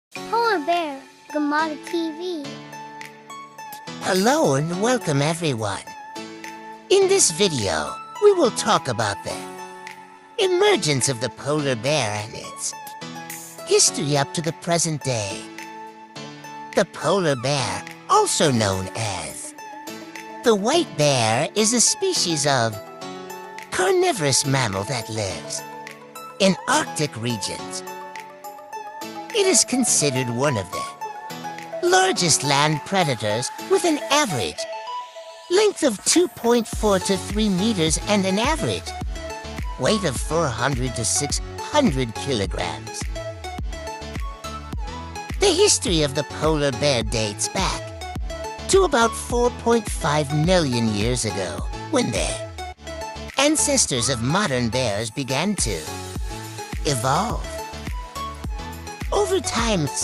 From Ancient Ancestors to Arctic Giants: The Fascinating Story of Polar Bears🐻‍❄️
The polar bear has a long and fascinating history that dates back millions of years. The ancestors of modern bears began to evolve and adapt to living in colder environments, eventually giving rise to the polar bear we know today. Throughout history, the polar bear has had a complex relationship with humans. Indigenous peoples of the Arctic, such as the Inuit, have a special connection with polar bears and have used them for food and materials for centuries. However, polar bear populations have also been heavily impacted by hunting and climate change, with melting sea ice making it increasingly difficult for polar bears to find food. Today, conservationists are working to protect this iconic species and ensure its survival for future generations.🐻‍❄️